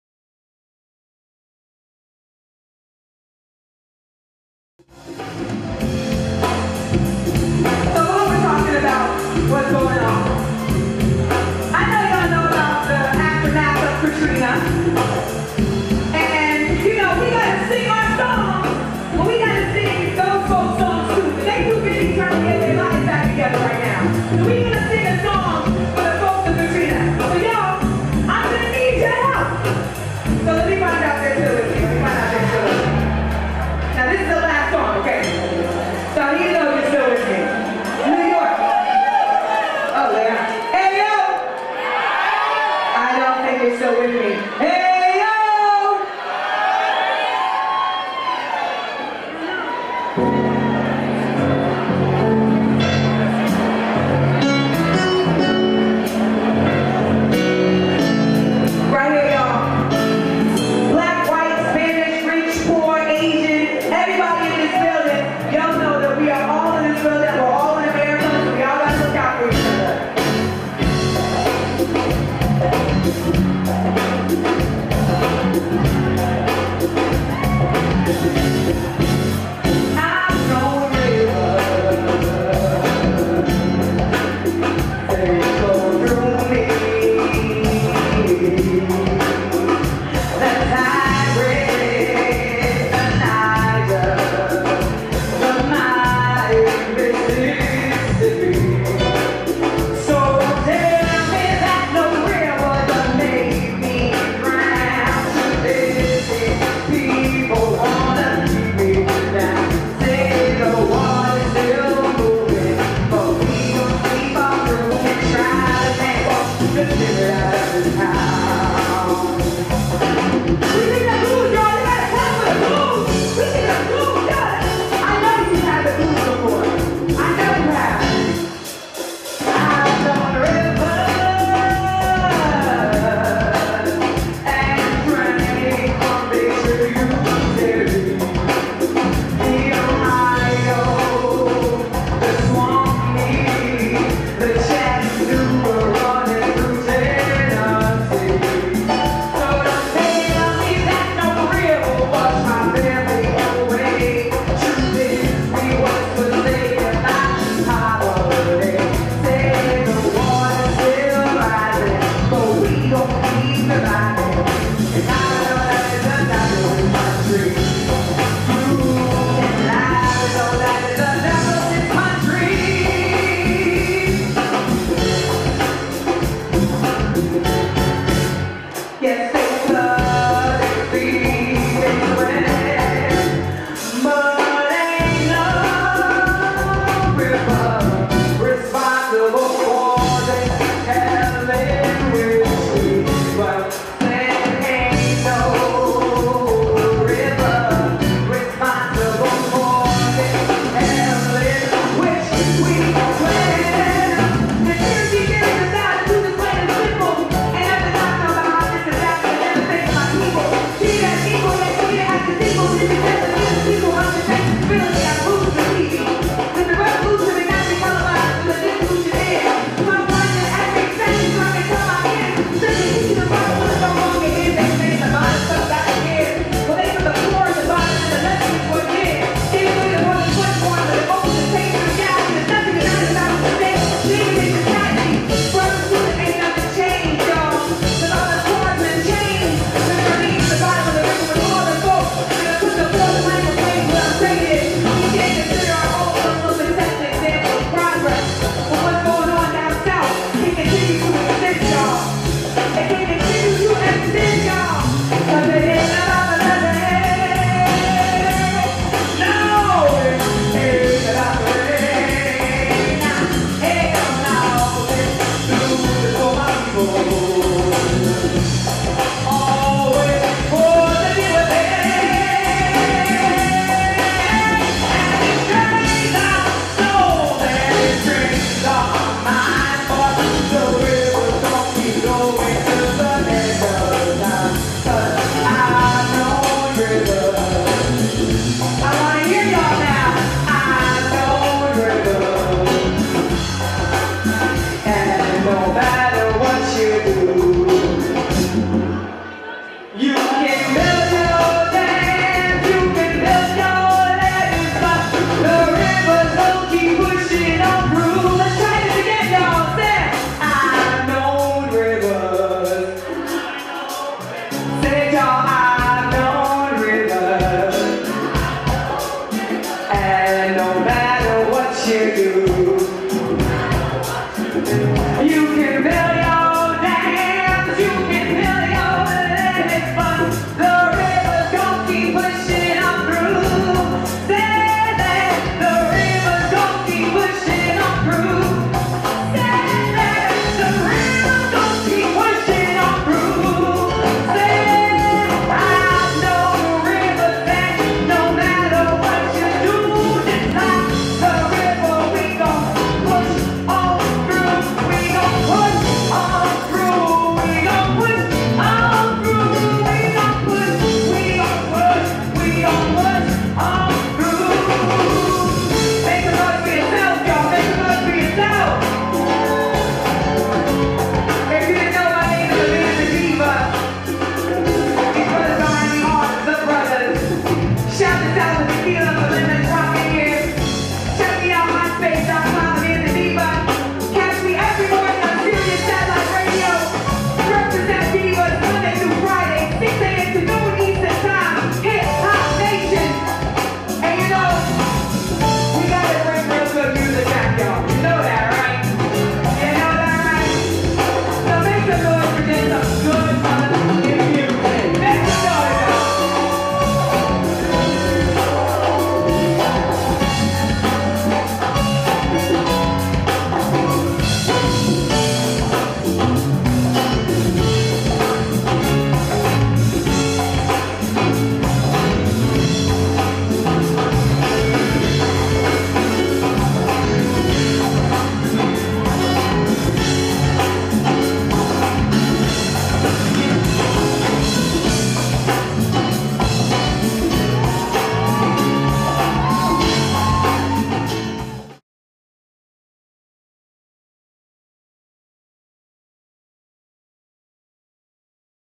Amanda Diva Hard Rock Live '06-Pt.3
part 3 of my show at hard rock cafe nyc with my band in oct '06